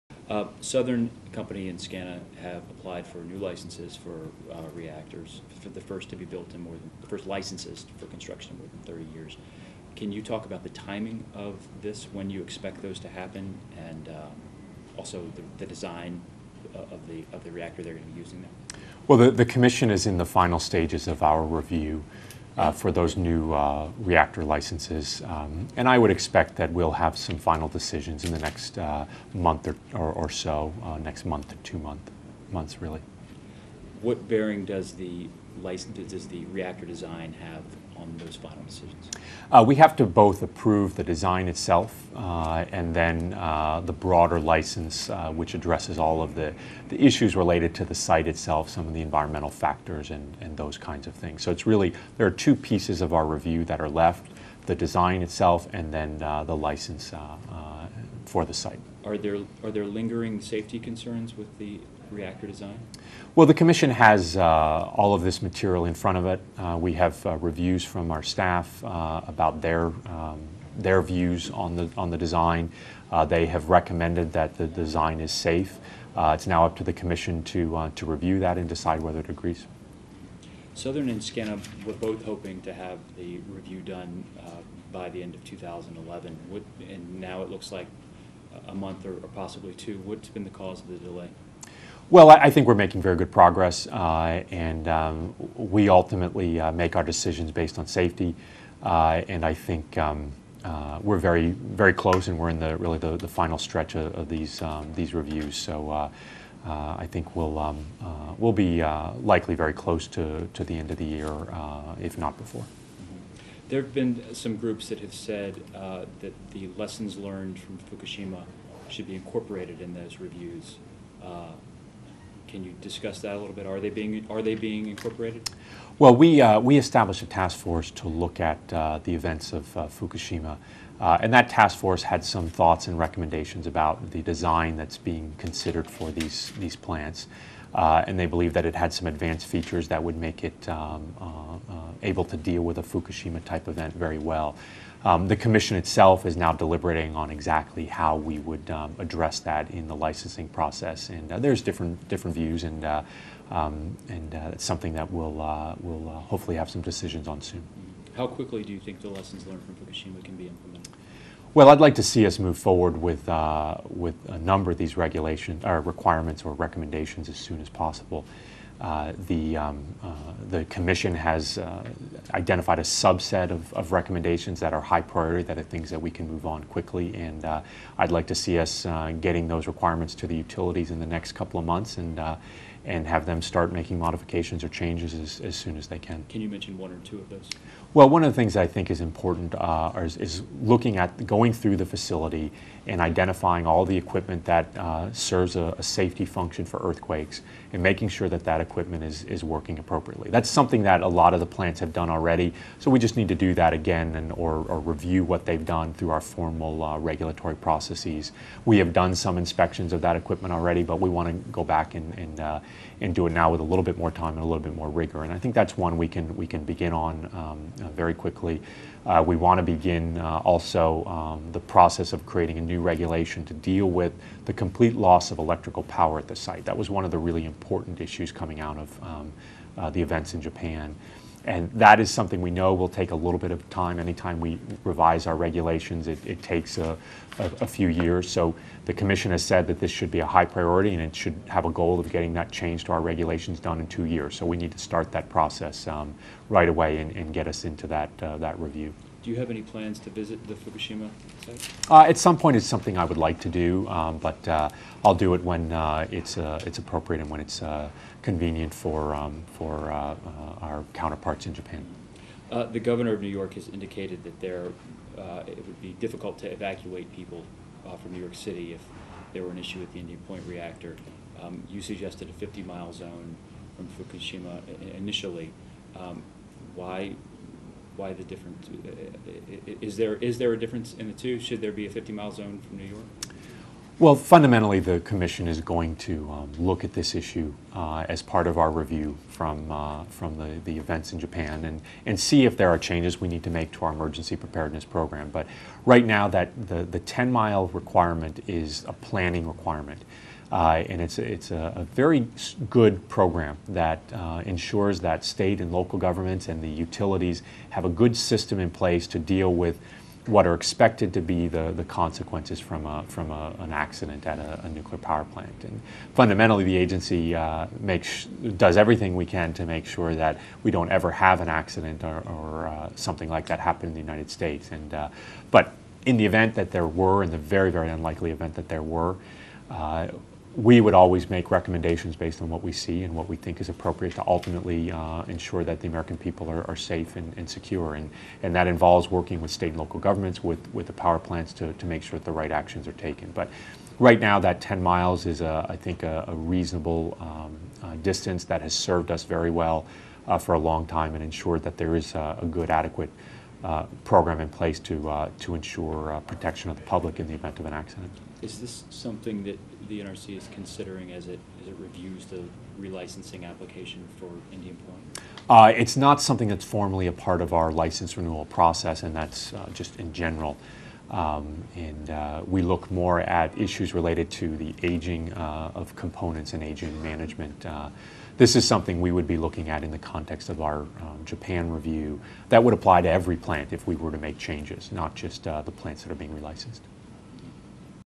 NRC's Jaczko on Nuclear Safety, New Reactors
Dec. 8 (Bloomberg) -- Gregory Jaczko, chairman of the U.S. Nuclear Regulatory Commission, talks with Bloomberg's Brian Wingfield about applications by Southern Co. and Scana Corp. to build new U.S. reactors, the agency's review of Tokyo Electric Power Co.'s Fukushima nuclear plant disaster and the outlook for regulations. (Source: Bloomberg)